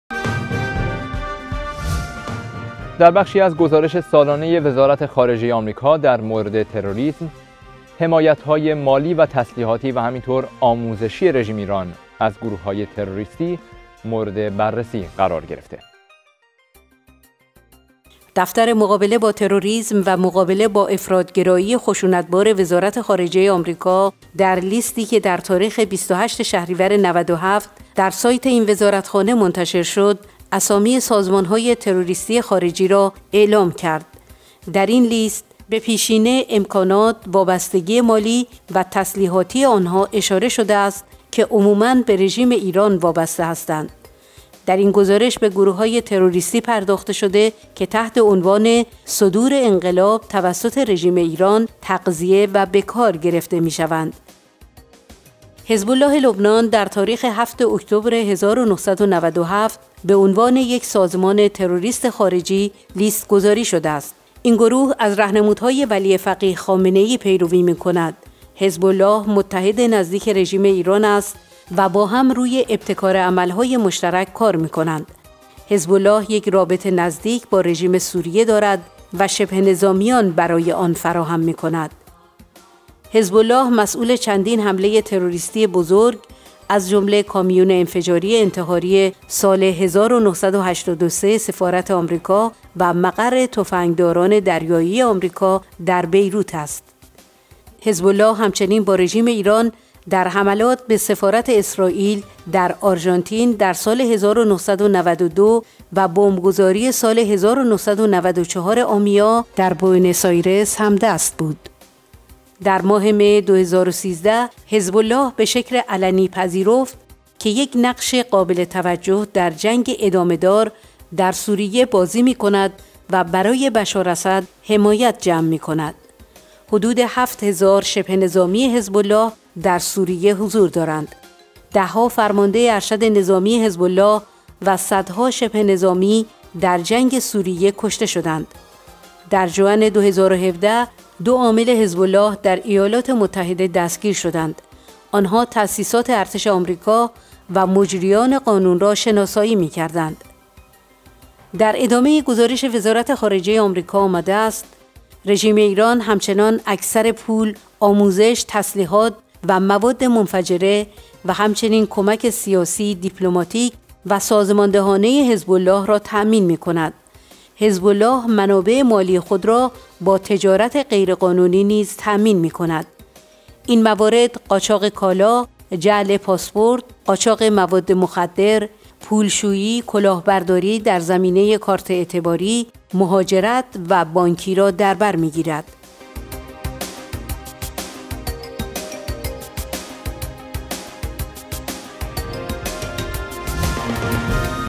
گزارش سالانه وزارت خارجه آمریکا -حمایت رژیم ایران از گروههای تروریستی
حمایت رژیم  ایران از گروههای تروریستی
در بخشی از گزارش سالانه وزارت خارجه آمریکا در مورد تروریسم، حمایت های مالی و تسلیحاتی و آموزشی رژیم ایران از گروههای تروریستی مورد بررسی قرار گرفته است:
دفتر مقابله با تروريسم و مقابله با افراط گرايي خشونت بار وزارت خارجه آمریکا در لیستی که در تاریخ ۲۸ شهریور ۹۷ در سایت این وزارتخانه منتشر شد اسامی سازمانهای تروریستی خارجی را اعلام کرد، در این لیست به پیشینه، امکانات ، وابستگی مالی و تسلیحاتی آنها اشاره شده است که عموما به رژیم ایران وابسته هستند. در این گزارش به گروههای تروریستی پرداخته شده که  تحت عنوان صدور انقلاب توسط رژیم ایران تغذیه و بکارگرفته میشوند:
 حزب الله لبنان  در تاريخ 7 اكتبر 1997 بعنوان يك سازمان تروريست خارجي ليست گذاري شده است. اين گروه از رهنمودهای ولی فقیه خامنه ای ، پيروی ميكند. حزب الله متحد نزديك رژیم ايران است و با هم روی ابتكار عملهای مشترك كار ميكنند. حزب الله يك رابطه نزديك با رژیم سوريه دارد و شبه نظامیان برای آن فراهم ميكند.
حزب الله مسئول چندين حمله تروريستي بزرگ از جمله كاميون انفجاری انتحاری سال 1983 سفارت آمريكا و مقر تفنگداران دريايي آمريكا در بيروت است؛ .
حزب الله همچنين با رژیم ايران در حملات به سفارت اسرائيل در آرژانتين در سال 1992 و بمبگذاري سال 1994 آميا در بوئنوس آيرس همدست بود.
در ماه مه 2013 حزب الله بشكل علني پذيرفت كه يك نقش قابل توجه در جنگ ادامه دار در سوريه بازی ميكند و برای بشار اسد حمايت جمع ميكند. حدود 7000 شبه نظامی حزب الله در سوريه حضور دارند؛ دهها فرمانده ارشد نظامي حزب الله و صدها شبه نظامی در جنگ سوريه كشته شده اند.
در ژوئن 2017 دو عامل حزب الله در ايالات متحده دستگير شدند. آنها تأسيسات ارتش آمريكا و مجريان قانون را شناسايي ميكردند.
 در ادامه گزارش وزارت خارجه آمریکا آمده است:
 رژیم ايران همچنان اكثر پول، آموزش، تسليحات و مواد منفجره و همچنين كمك سياسی، ديپلماتيك و سازماندهانه حزب الله را تأمين ميكند. حزب الله منابع مالی  خود را با تجارت  غیرقانونی نیز تأمین می کند. اين موارد قاچاق كالا، جعل پاسپورت، قاچاق مواد مخدر، پولشويی، كلاه برداری در زمينه كارت اعتباری، مهاجرت و بانكي را در برميگيرد.